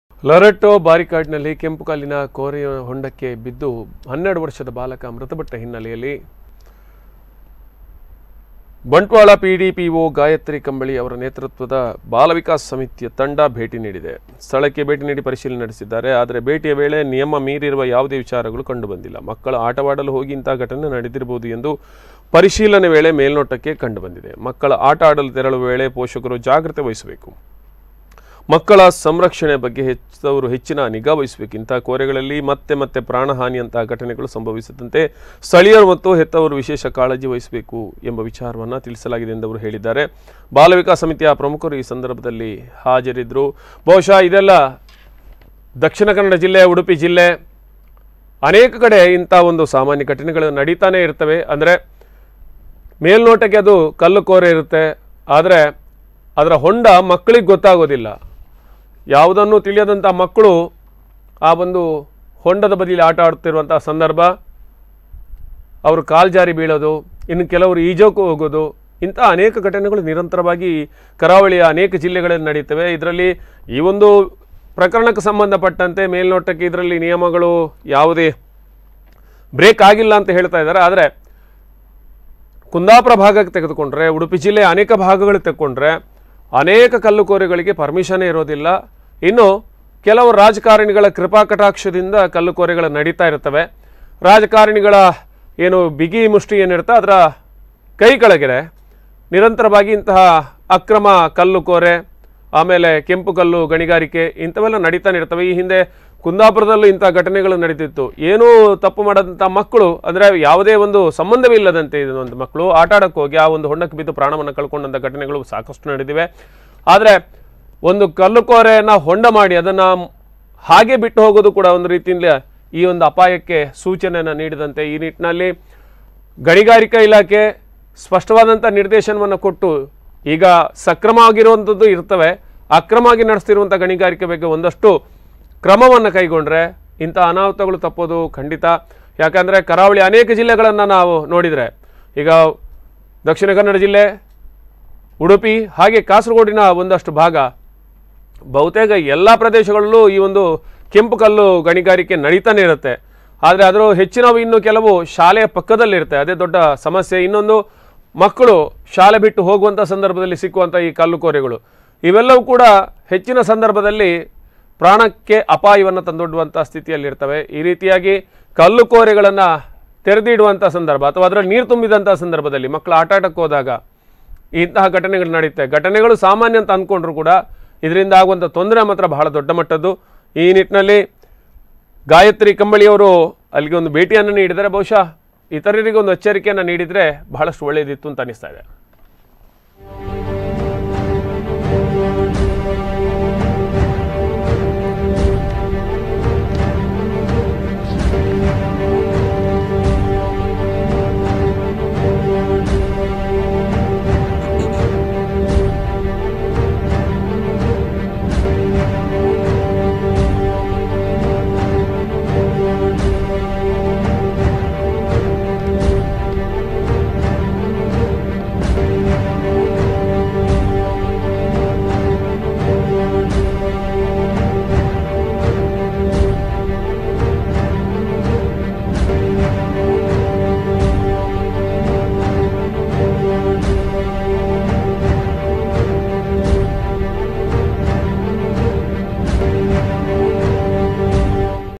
ಕೆಂಪುಕಲ್ಲು ಕೋರೆಗೆ ಬಾಲಕ ಬಿದ್ದು ಮೃತಪಟ್ಟ ಪ್ರದೇಶಕ್ಕೆ ಬಂಟ್ವಾಳ ಸಿ.ಡಿ.ಪಿ.ಒ.ಗಾಯತ್ರಿ ಕಂಬಳಿ ಭೇಟಿ
ಲೊರೆಟ್ಟೋ ಬಾರಿಕ್ಕಾಡಿನಲ್ಲಿ ಕೆಂಪುಕಲ್ಲಿನ ಕೋರೆಯ ಹೊಂಡಕ್ಕೆ ಬಿದ್ದು 12 ವರ್ಷದ ಬಾಲಕ ಮೃತಪಟ್ಟ ಹಿನ್ನೆಲೆಯಲ್ಲಿ ಬಂಟ್ವಾಳ ಸಿ.ಡಿ.ಪಿ.ಒ.ಗಾಯತ್ರಿ ಕಂಬಳಿ  ಅವರ ನೇತೃತ್ವದ , ಬಾಲವಿಕಾಸ ಸಮಿತಿಯ ತಂಡ ಬೇಟಿ ನೀಡಿದೆ ಸ್ಥಳಕ್ಕೆ ಭೇಟಿ